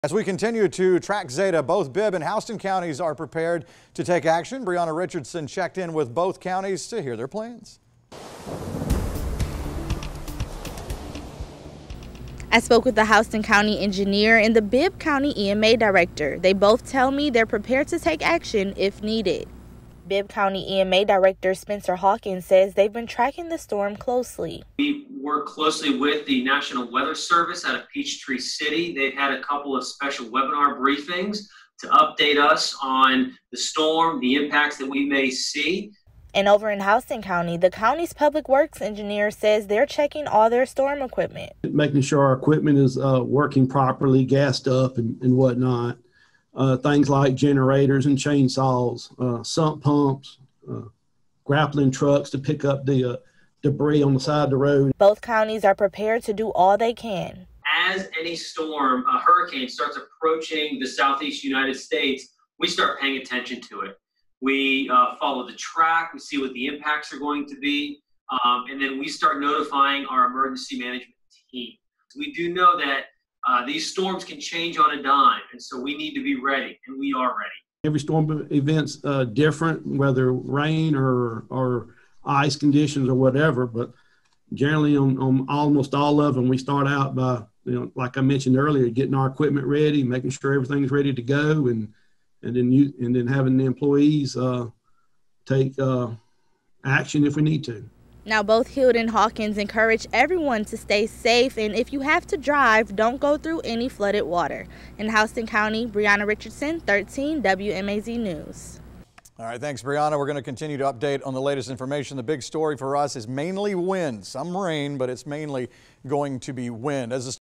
Bibb, Houston counties prepare for impact of Hurricane Zeta
As we continue to track Hurricane Zeta, both Bibb and Houston counties are prepared to take action.

Bibb County EMA Director Spencer Hawkins said they've been tracking the storm closely.

"We work closely with the National Weather Service at Peachtree City, they've had a couple of special webinar briefings to update us on the storm, the impacts that we may see," said Hawkins.

Houston County's Public Works' Engineer Ronnie Heald said they are checking all their storm equipment.

"Making sure our equipment is working properly gassed up and whatnot, things like generators and chainsaws, sump pumps, grappling trucks to pick up the debris on the side of the road," said Heald.

13WMAZ News is the #1 station for news and weather in the Central Georgia area, serving people in Dublin, Macon, Milledgeville and Warner Robins.